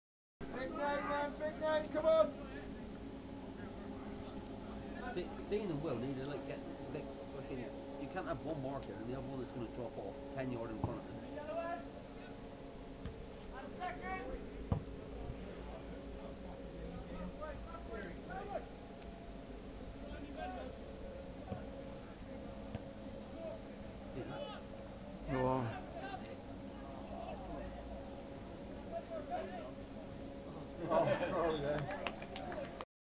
Duchy Premier League Game, St Stephen 1st Team 8 - 2 St Dominick
Craig Coad gets his Hat-Trick on 58 minutes when Chris Kellow passes to him in the box.